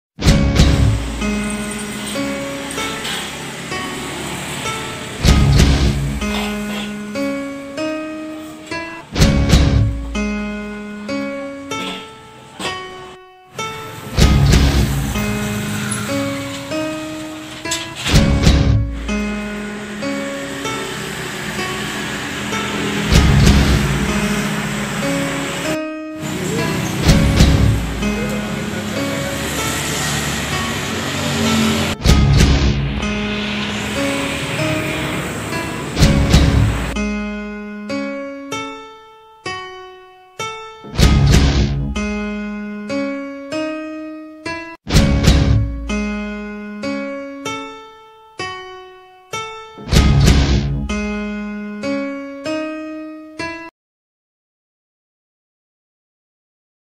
Memasang Neonbox Besar
memasang Neonbox Besar di kota Gemolong, Sragen